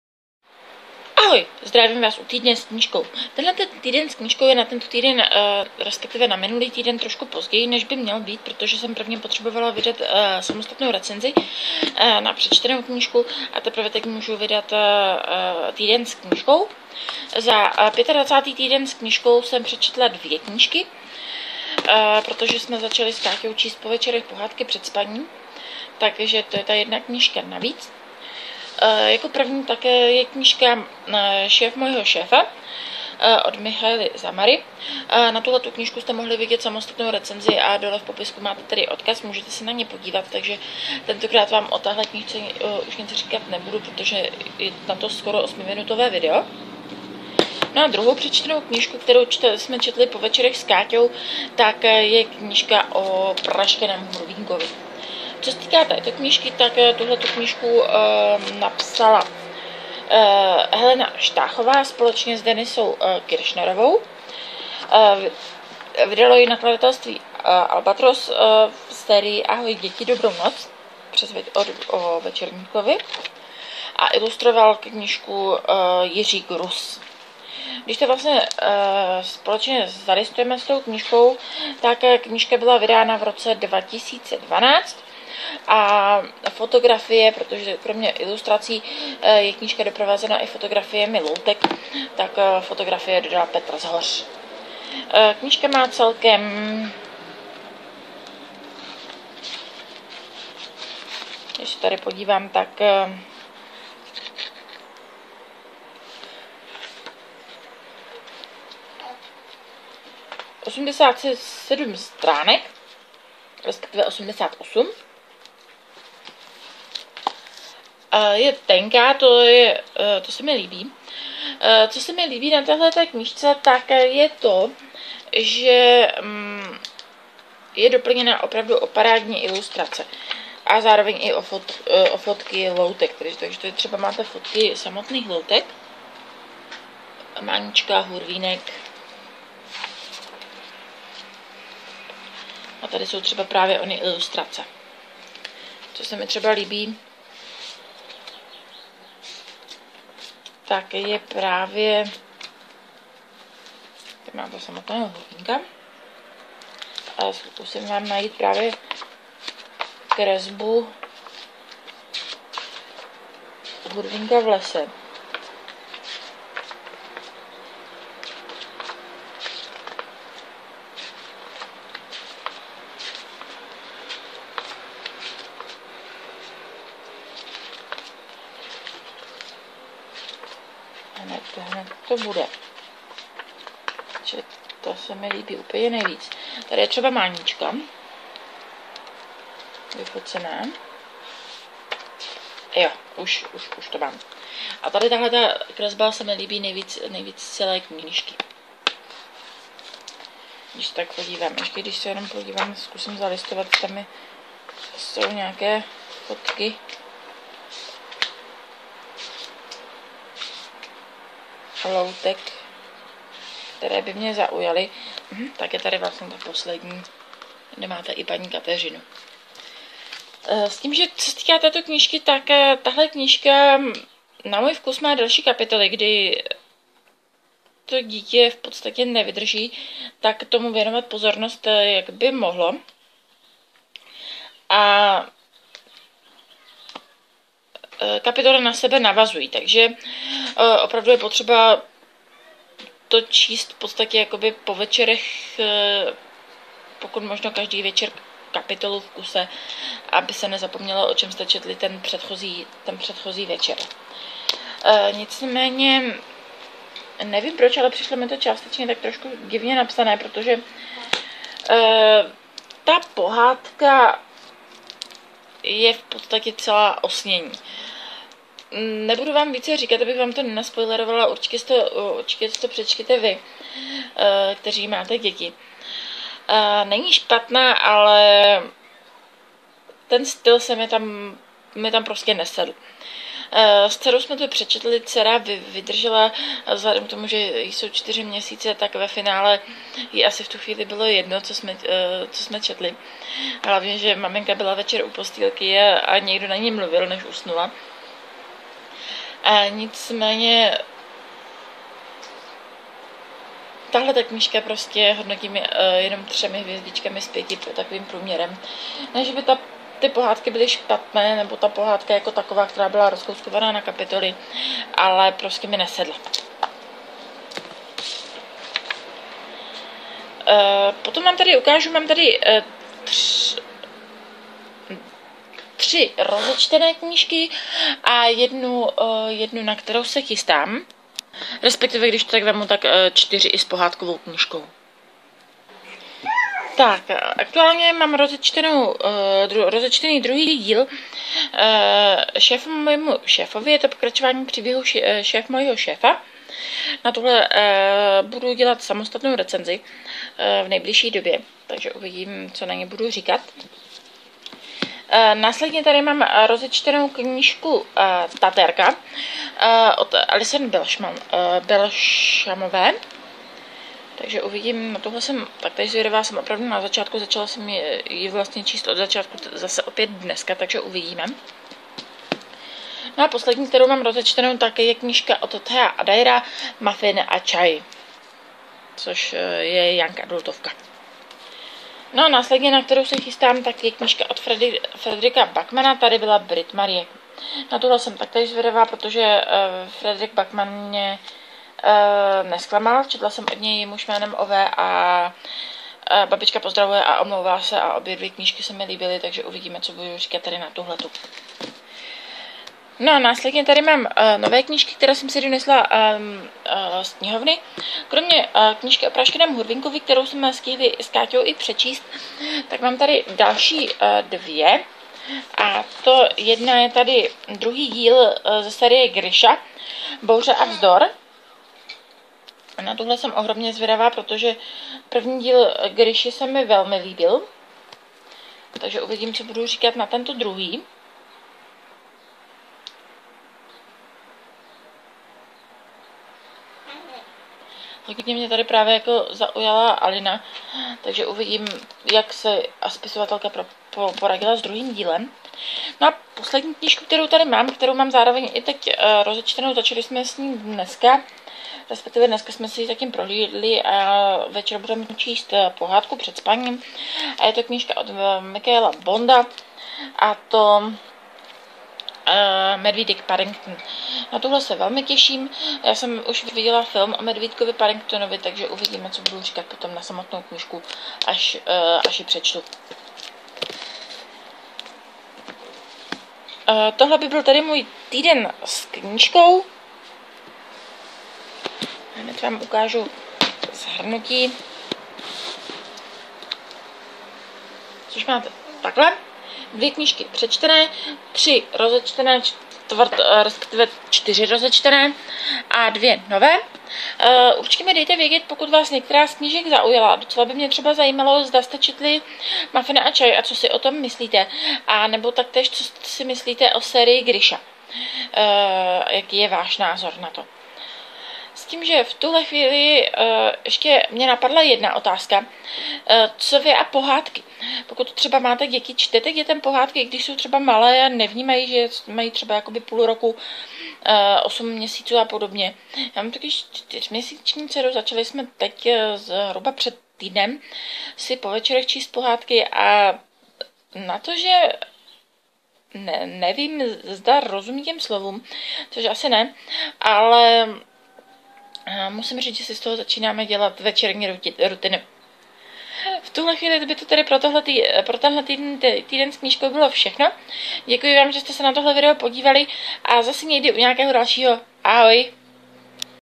Týden s knížkou 25/2019
Ahoj přátelé a milovníci knih,
vítám Vás u dalšího týdne s knížkou. Jde tentokrát o shrnutí 25. týdne roku 2019.
Knihy, o kterých mluvím:
- Michaela Zamari - Šéf mojho šéfa,
- Denisa Kirschnerová a Helena Štáchová - O praštěném Hurvínkovi,
- Michaela Zamari  - Šéfom mojmu šéfovi,
- Alison Belsham - Tatérka,
- Theodor Addair - Muffin a čaj,
- Fredrick Backman - Tady byla Britt Marie,
- Leigh Bardugo - Griša II. Bouře a vzdor,
- Michael Bond - Medvídek Paddington,